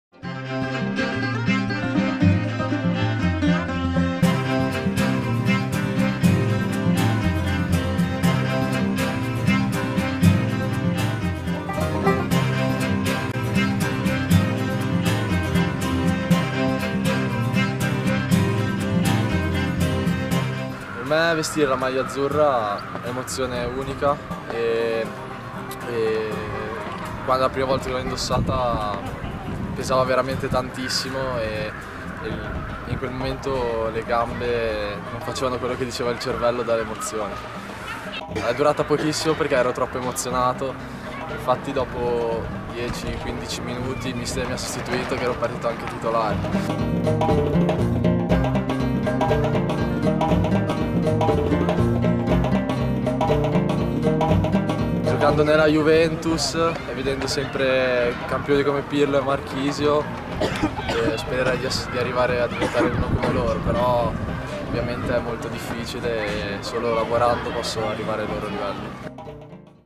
Emozioni Azzurre - Mattia Vitale
Aneddoto particolare, legato al ricordo più intenso e decisamente emozionante, del giovane centrocampista della Juventus sul suo esordio in Under 15...